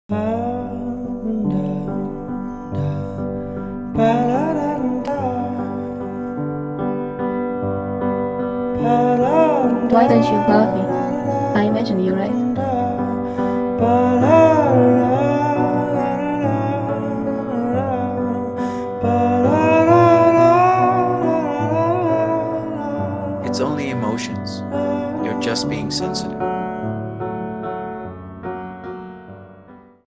PYGMALION Trailer
Collaboration with Dasha Sandulenko.
She asked me to help her with a school project (make a movie trailer for a movie that never existed) and this is what happened.

Tell me what you think 😊 should we make the movie?